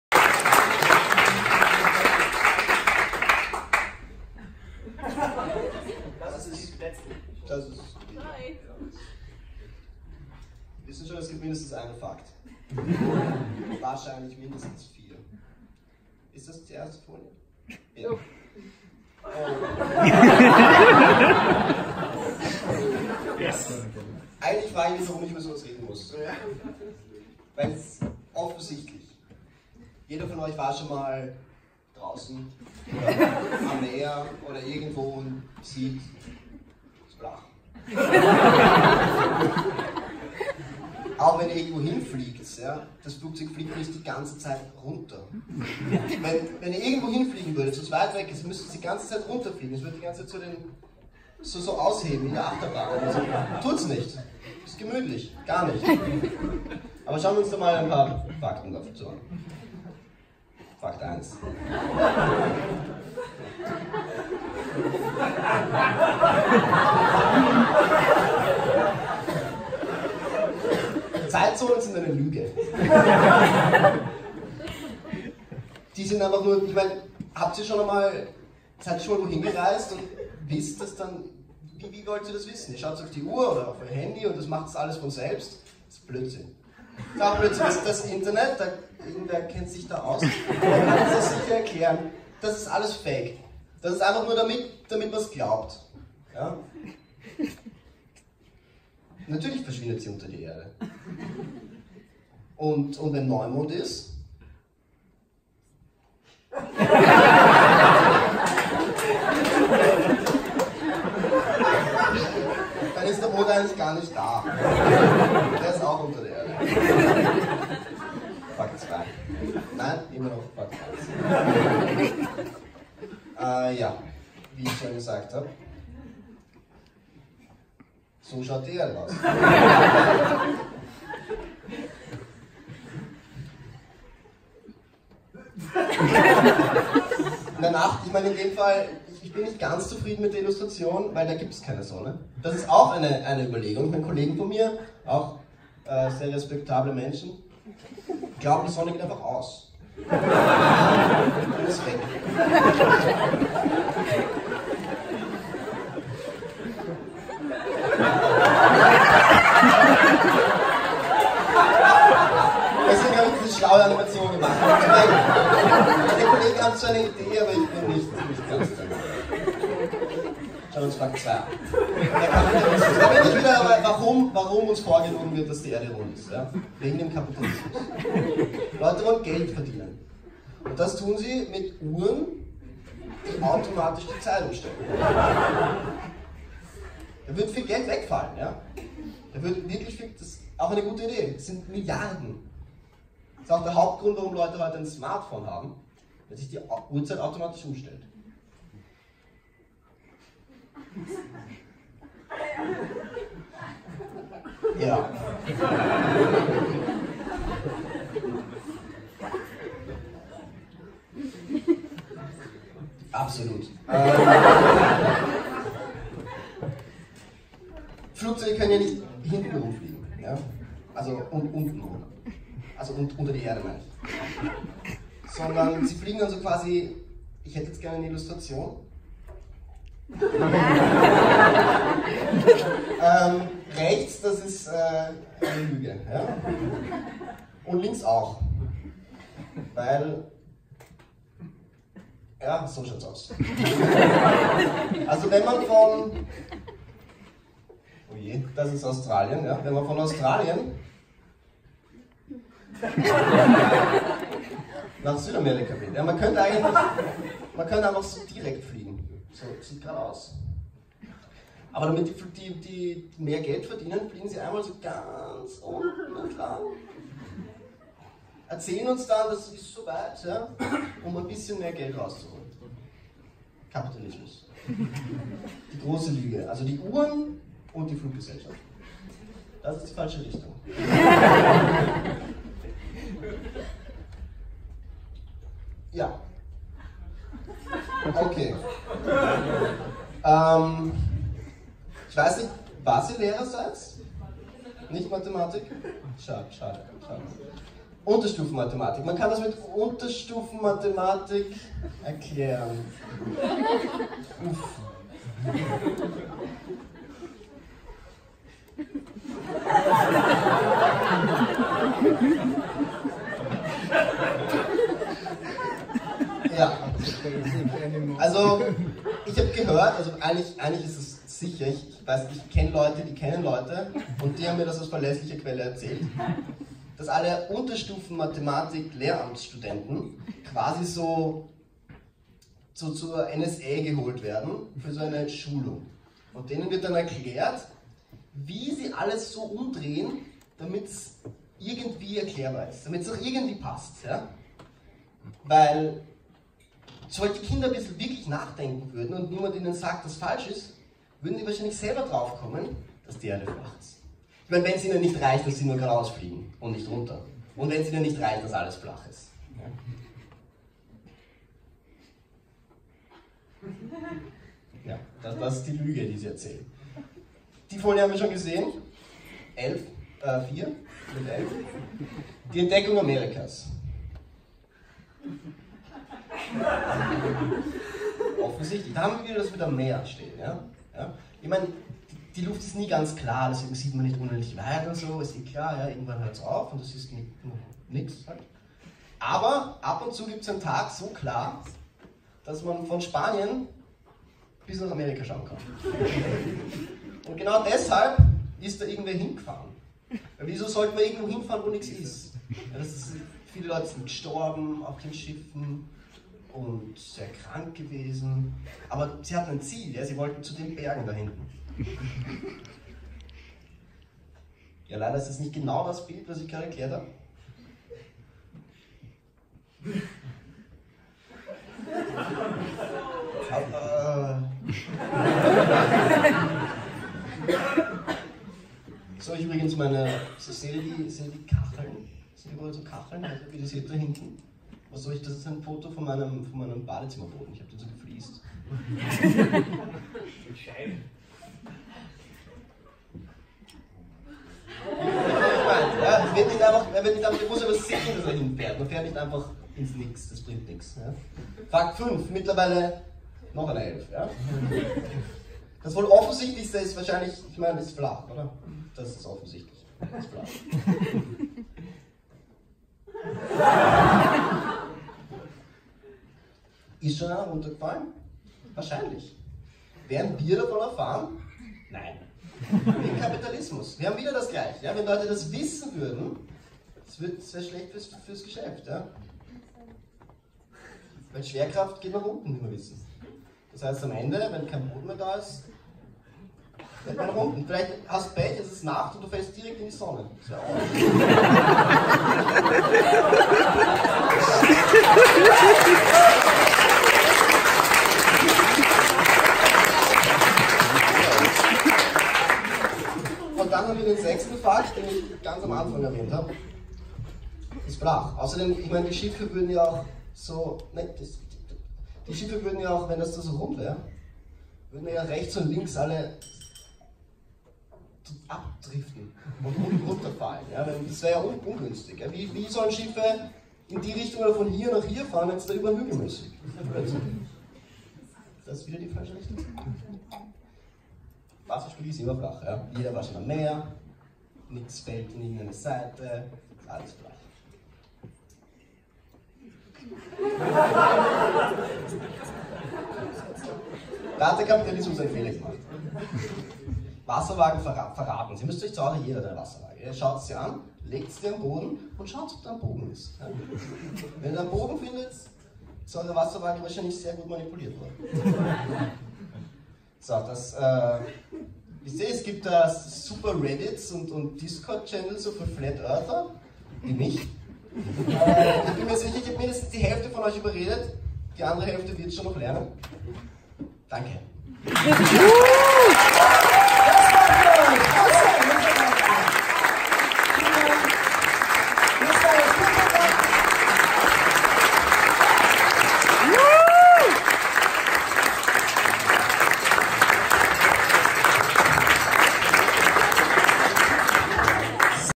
Powerpoint karaoke - FOMP im Spektakel - Thema: Flat Earth
Powerpoint Karaoke abend im Spektakel!

Improvisierte Präsentation, hatte keine Idee was ich präsentieren muss, keine Folie war mir bekannt.